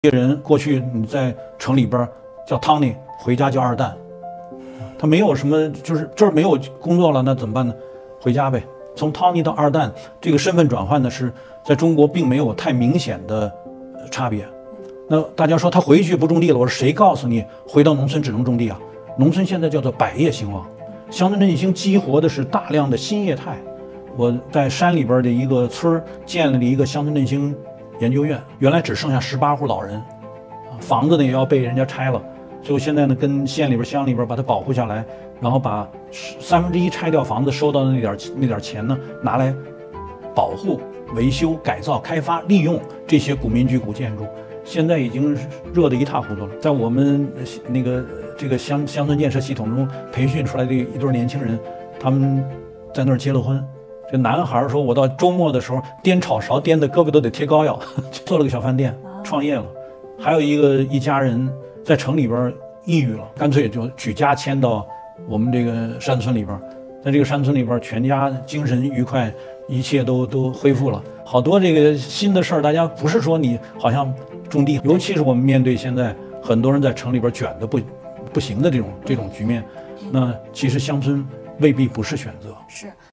温铁军：从Tony到“二蛋”，其实不用非得在城里“卷”
在城里没了工作怎么办？回家呗。这些人过去在城里叫Tony，回家叫二蛋，从Tony到二蛋这种身份转换，在中国并没有太大差别。大家说他回去不就是种地吗？谁告诉你回到农村只能种地？农村现在百业兴旺，乡村振兴激活的是大量新业态。